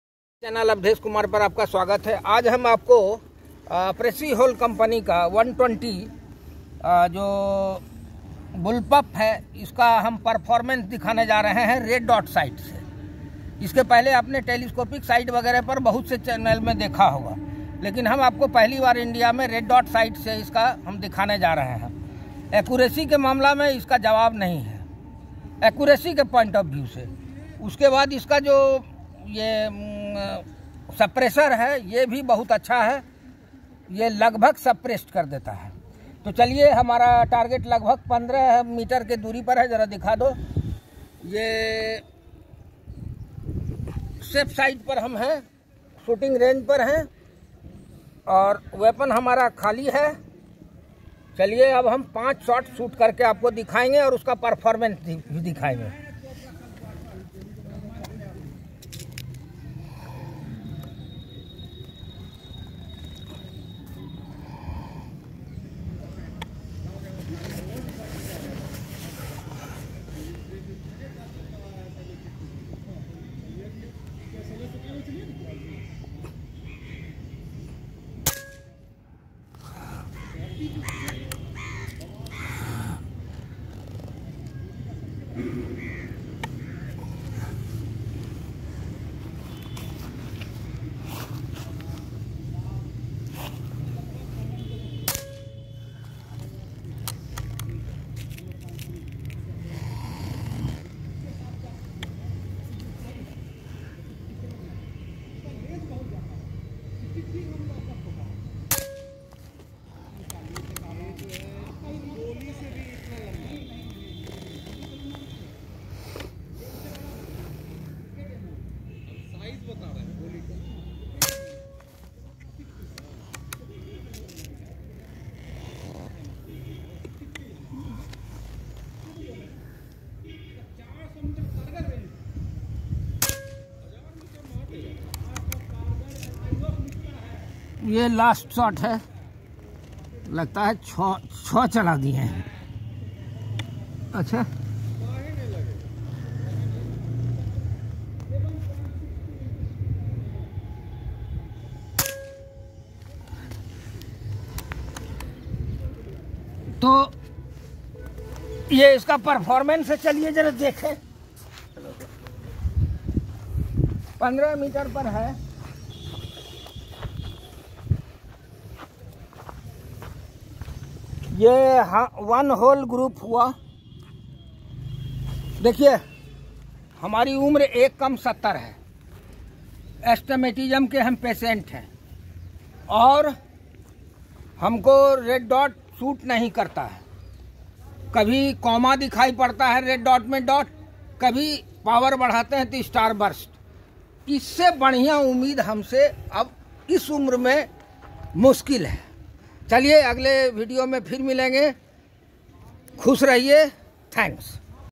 Performance of precihole 120 Minotaur in shooting Range
Accuracy  test of 120 Minotaur